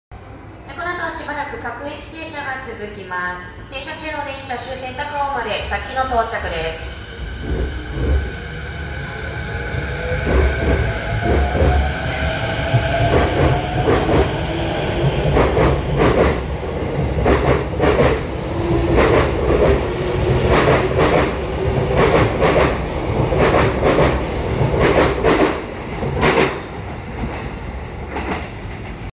●20190502 E233系 発車 ＠東京２
撮影路線：JR東日本 中央東線
撮影駅：東京
行先：中央特快 高尾
車両：E233系
両数：10両
撮影機材：【スマホ】SONY Xperia XZ2 Premium
MH：ーー
備考欄：中央線開業130周年記念ラッピング編成です